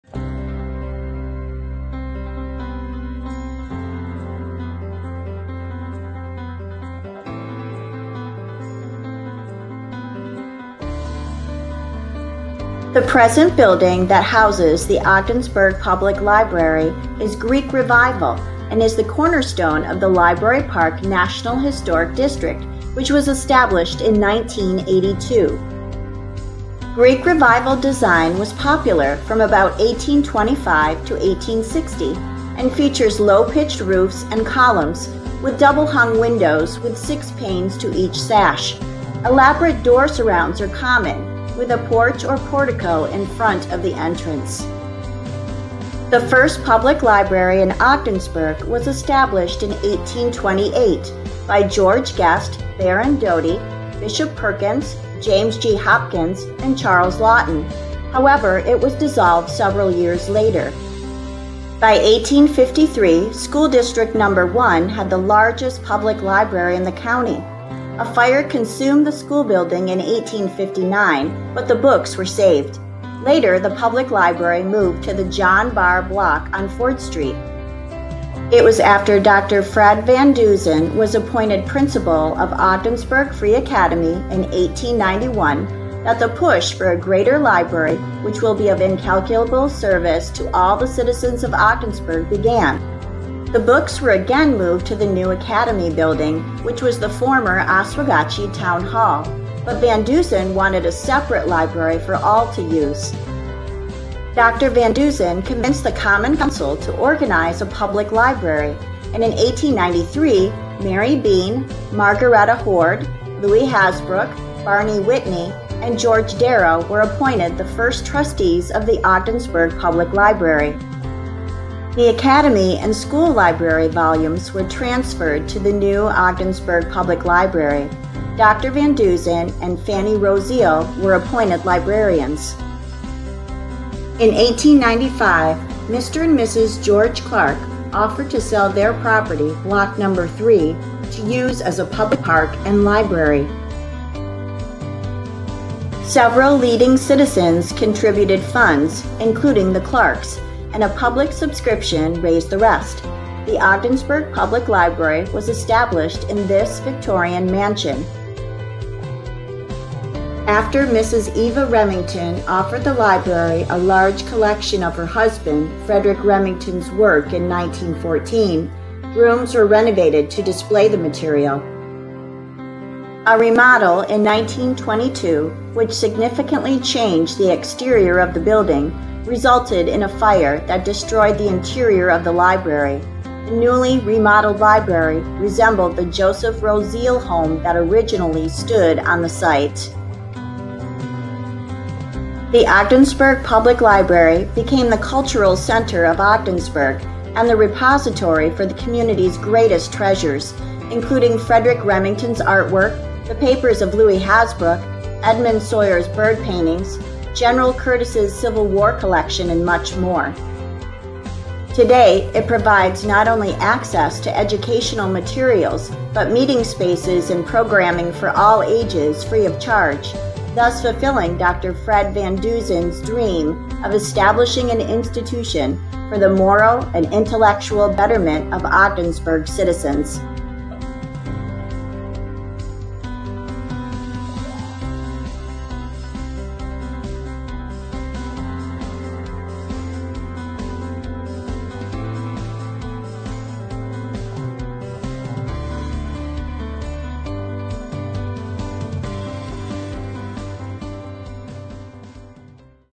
History of the Ogdensburg Public Library
The present building that houses the Ogdensburg Public Library is Greek Revival and is the cornerstone of the Library Park National Historic District, which was established in 1982. Today it provides not only access to educational materials but meeting spaces and programming for all ages free of charge thus fulfilling Dr. Fred Van Dusen’s dream of establishing an institution for the moral and intellectual betterment of Ogdensburg’s citizens.